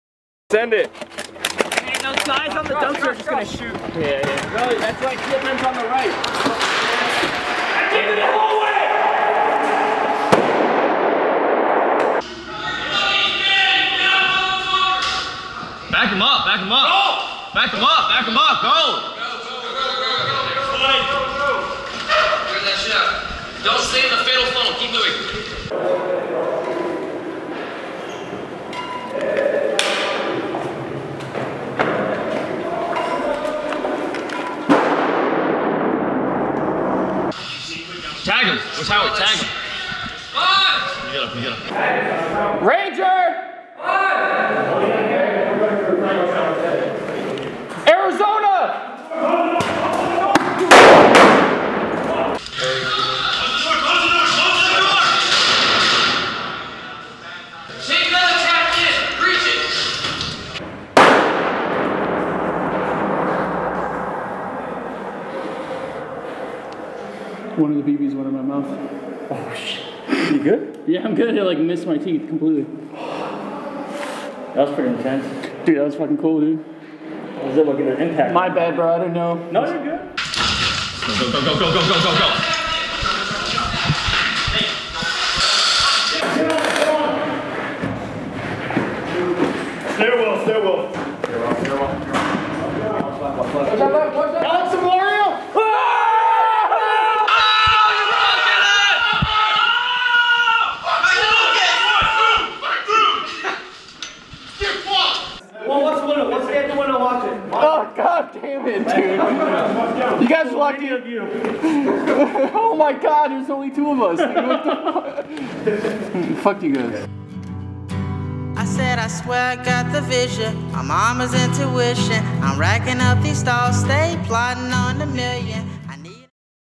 MSW - Caspian Breakout - NATO VS RU (TFM & Snakes)
During the Caspian Breakout, there was one instance where members of ST04 met on the field as enemies (Taskforce Mayhem vs Snake Platoon)

Instagram.com/spartanteam04
Facebook.com/spartanteam04